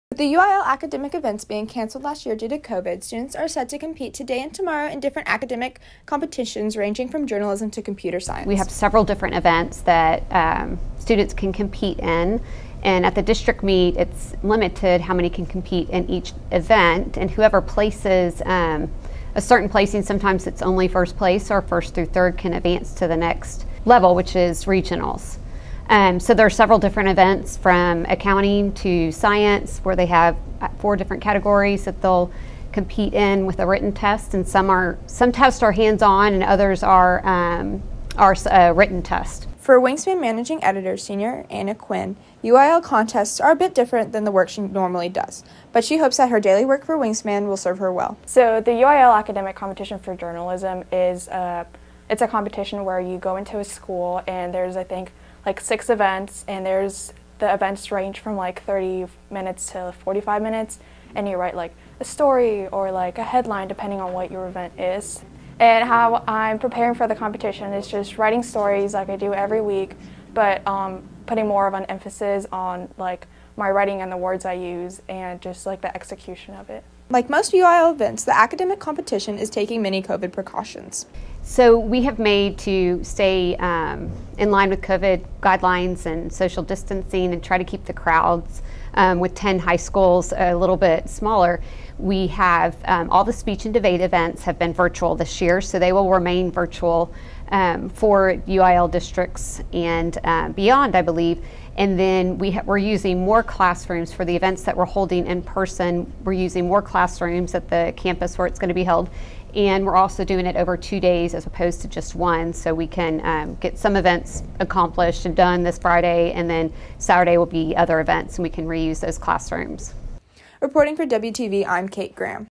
UIL Academic Meet
WTV's Kate Graham has the details about the UIL Academic meet being held at Centennial High School.